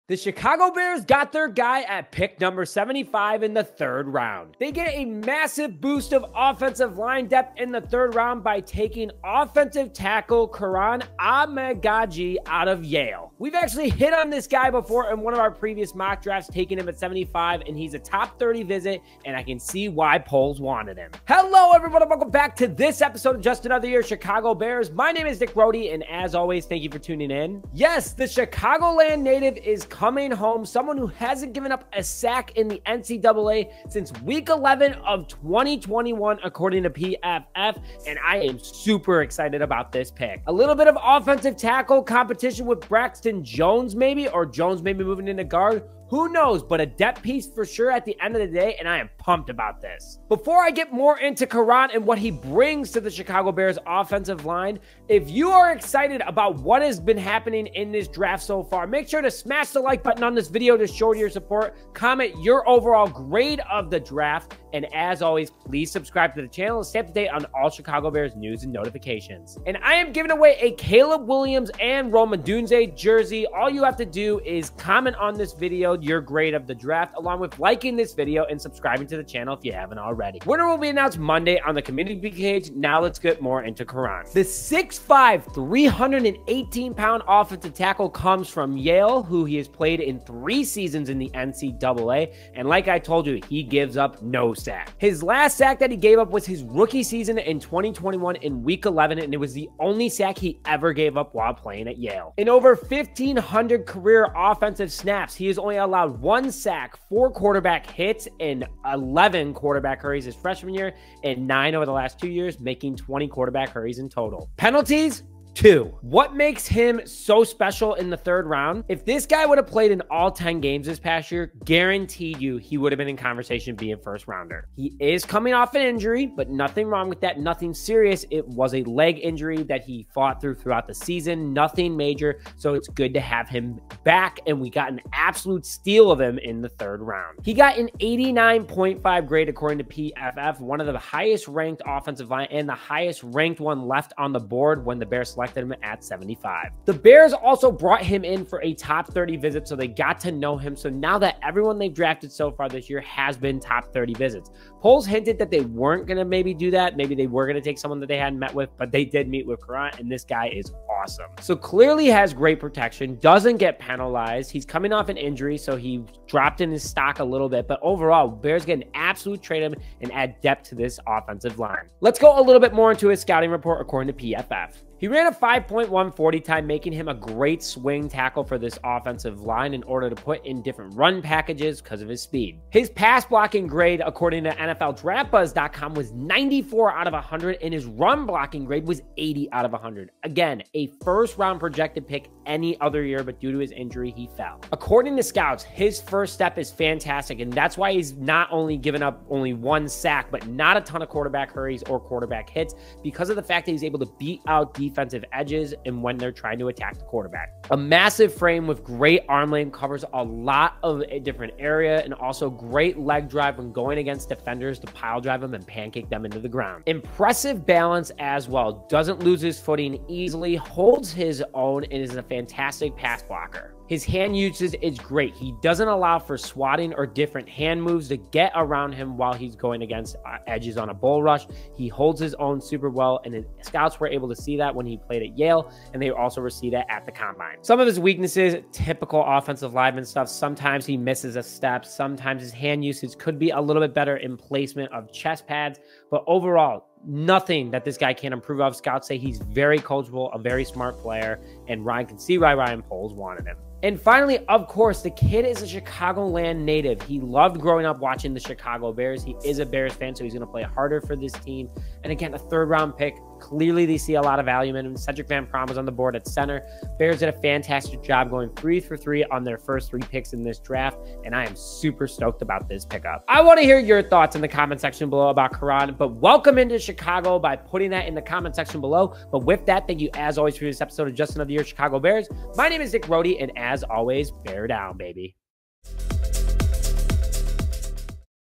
Bears LAND MAJOR STEAL At #75 w/Yale OT Kiran Amegadjie. MASSIVE UPGRADE To Offensive Line
The Chicago Bears land a MAJOR STEAL in Yale OT Kiran Amegadjie at pick #75.